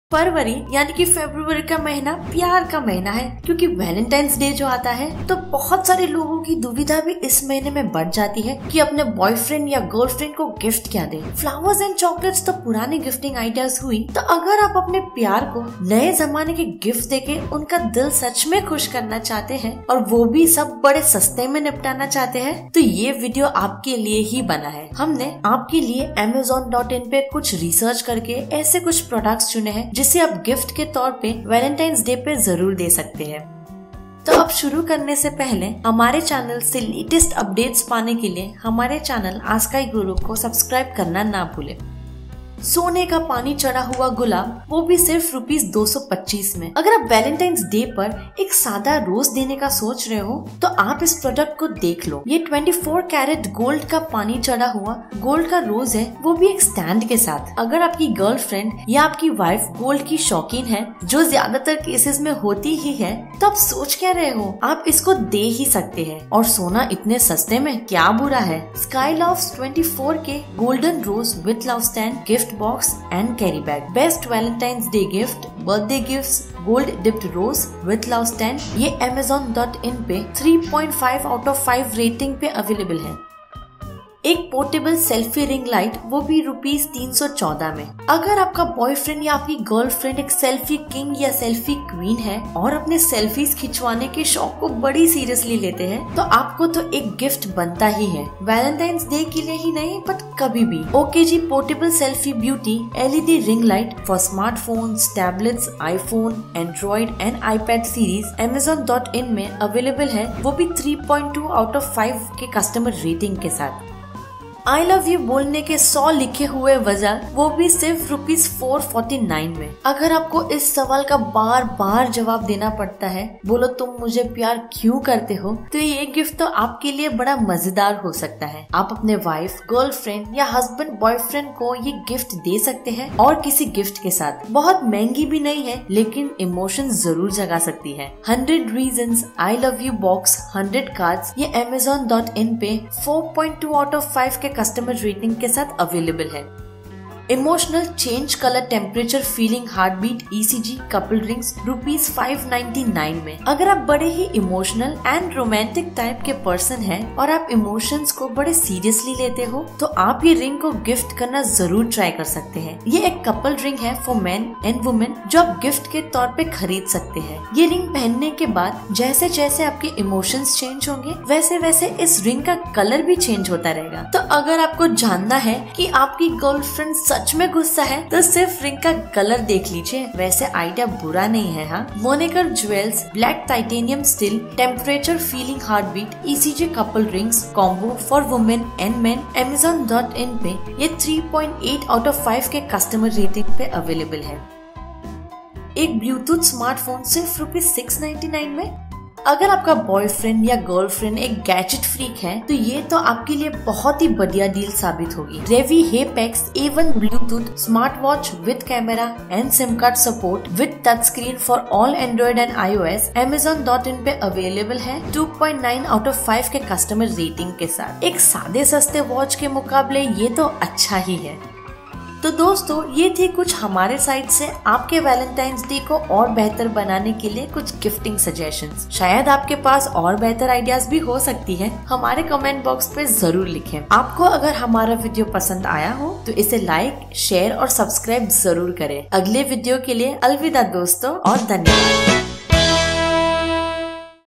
GIFT TO GIVE ON VALENTINES DAY | वैलेंटाइन डे के लिए 5 GIFT IDEAS -AMAZING VALENTINES DAY GIFT IDEAS
GIFT TO GIVE ON VALENTINES DAY | वैलेंटाइन डे के लिए 5 GIFT IDEAS -AMAZING VALENTINES DAY GIFT IDEAS - ASKIGURU

फ़रवरी (फेब्रुवरी)  का महीना यानी प्यारा का महीना, वॅलिंटाइन्स दे जो आता है….तो बोहोट सारे लोगों की दुविधा भी इश्स महीने में बढ़ जाती है के अपने गर्लफ्रेंड का बाय्फ्रेंड को गिफ्ट क्या दे….फ्लवर्स आंड चॉक्लेट्स तो पुरानी गिफ्टिंग आइडियास हुई….तो अगर आप अपने प्यार को नये जमाने के गिफ्ट्स देके उनका दिल सच में खुश करना चाहते है और हू भी बड़े सस्ते में यह निपटना चाहते है….तो यह वीडियो आपके लिए ही बना है.
हुँने आपके लिए आमज़ॉन.इन पे कुछ रिसर्च करके ऐसे कुछ प्रॉडक्ट्स चुने है ….जिससे आप गिफ्ट के तौर पे…..वॅलिंटाइन्स दे पे ज़रूर दे सकते है.
तो चलिए अब हम बढ़ते है अपने वीडियो की और.